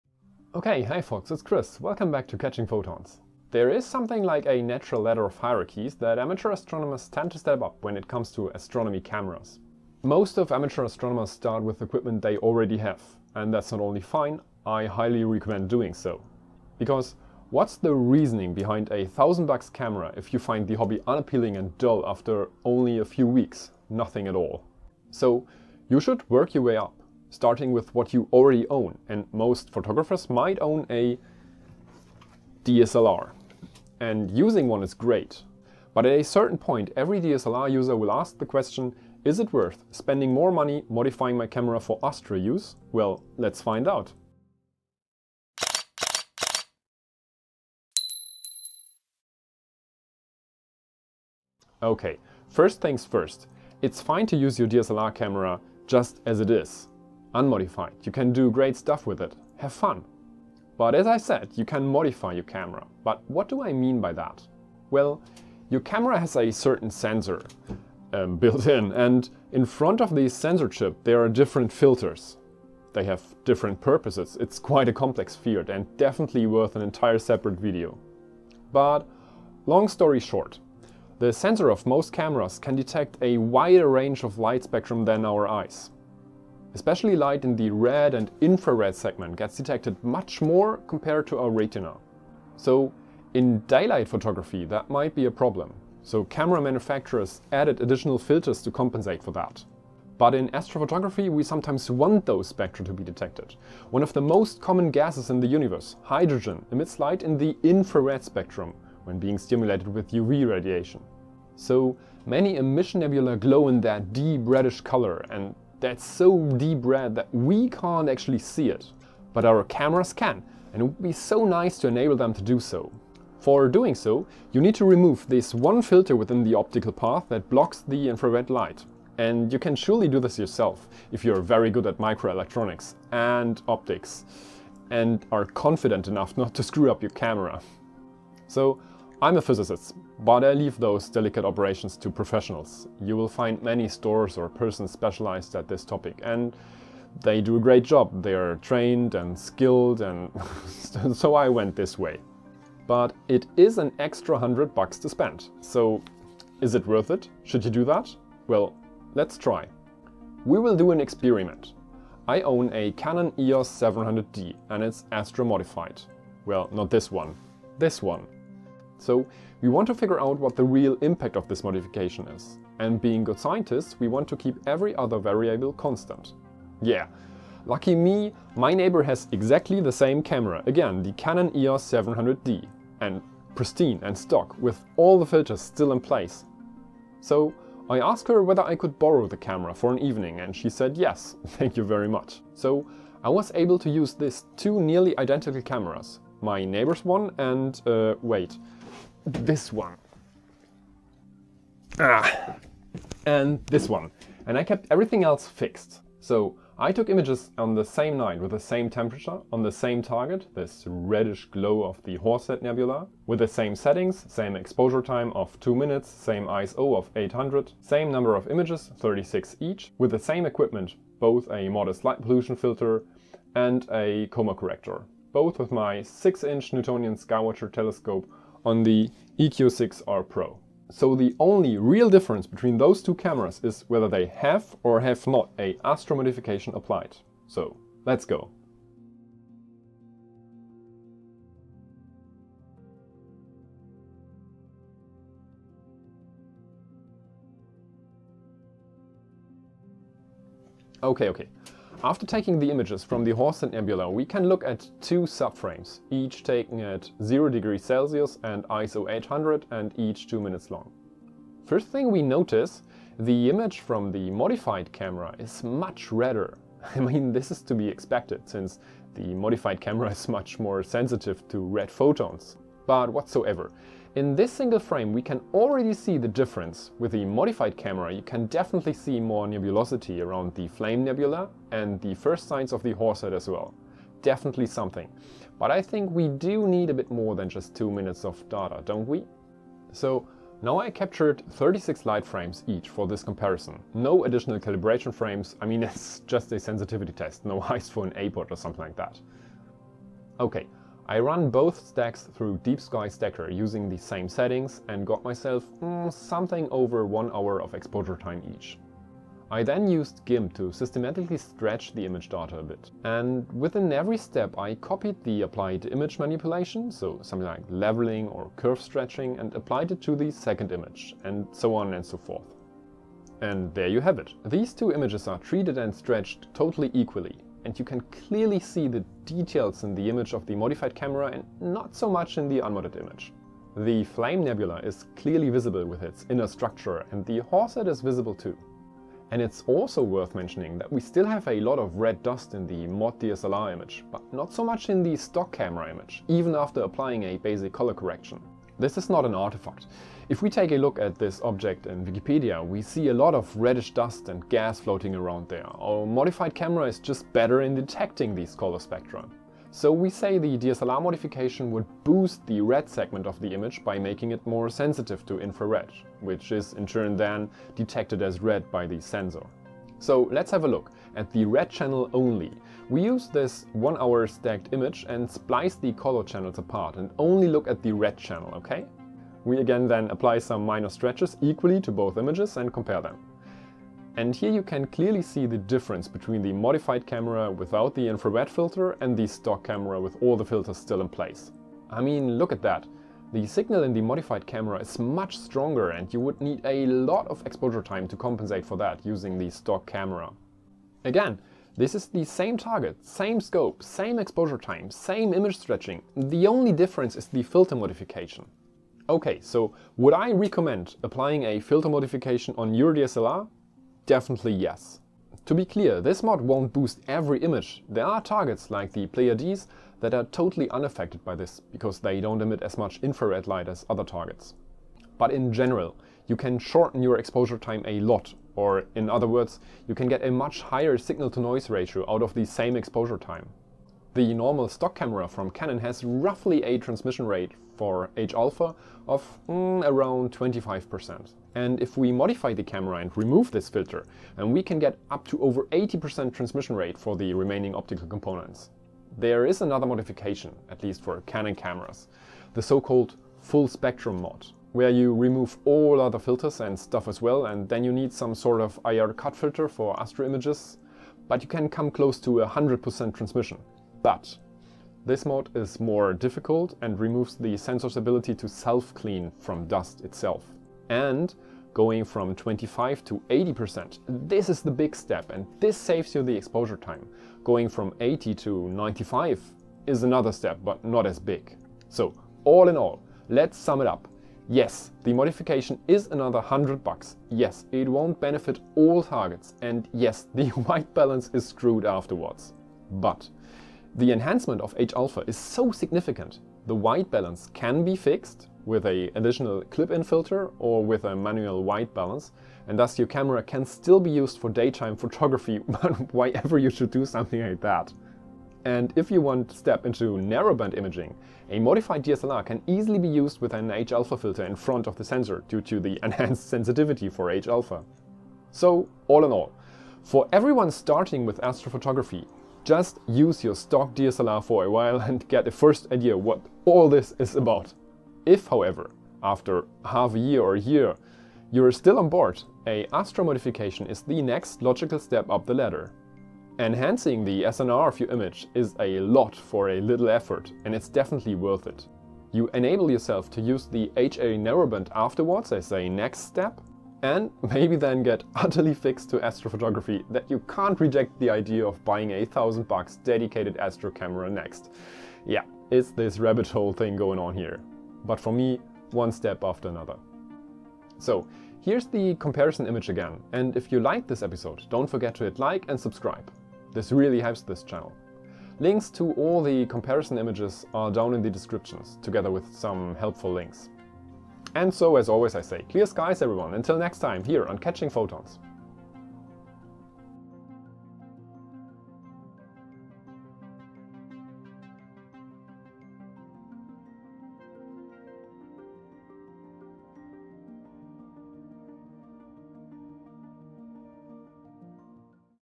Astro Tutorial #2.9.2: DSLR Astro - Modification Comparison (Stock vs. Mod Canon EOS700D)
Hey folks,
in this video we are going to investigate the effect of the DSLR Astro Modification. Therefore we are going to compare two nearly identical cameras: The Canon EOS 700D Stock vs. Canon EOS 700D astromodified

We will discuss the effects of the modification, dig in some theory and highlight the pros and cons of modifying your DSLR.

I hope this episode can give you a good insight into camera modification.

Clear skies and till next time here on catchingphotons!
-Chris